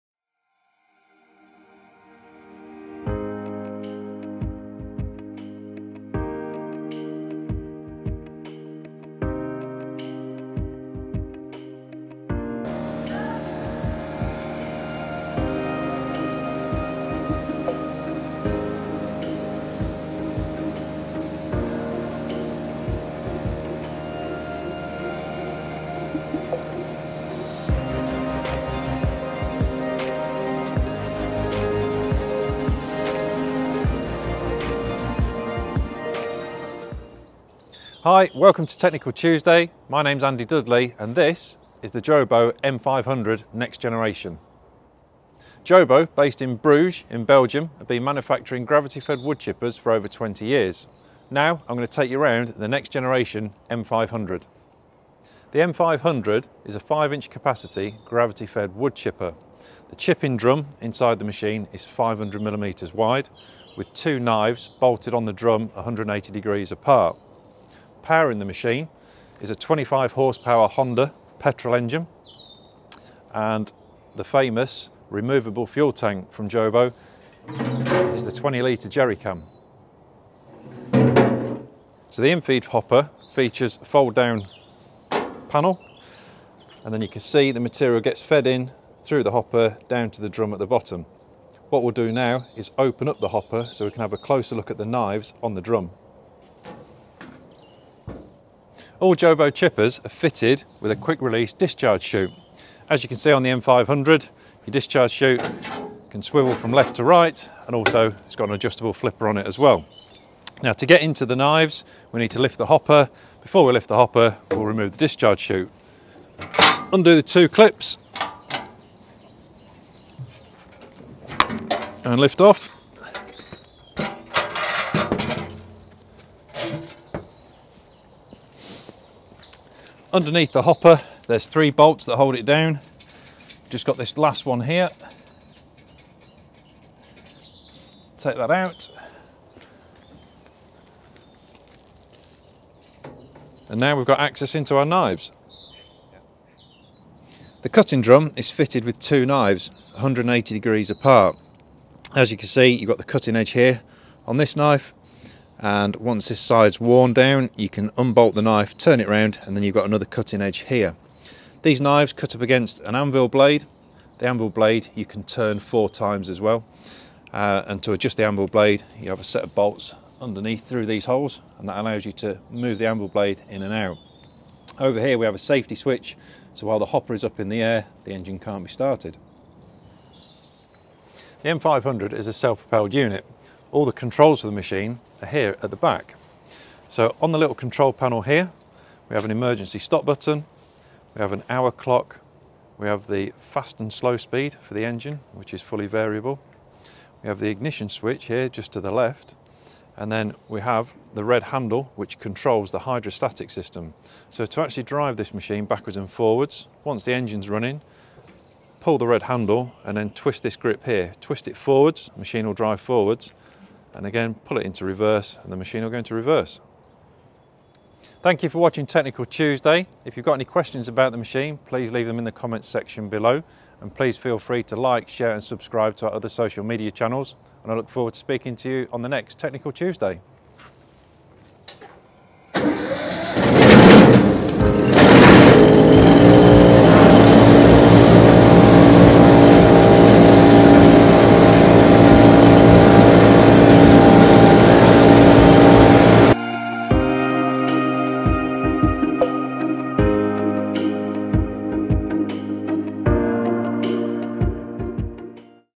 Global Recycling Solutions Ltd - Technical Tuesday Jo Beau M500 Next Generation Wood Chipper
Join Andy Dudley, Technical Consultant, as he shows you around the Jo Beau M500 Next Generation Wood Chipper in this episode of Technical Tuesday.

Please leave any questions in the comments section below and Andy will be happy to answer them.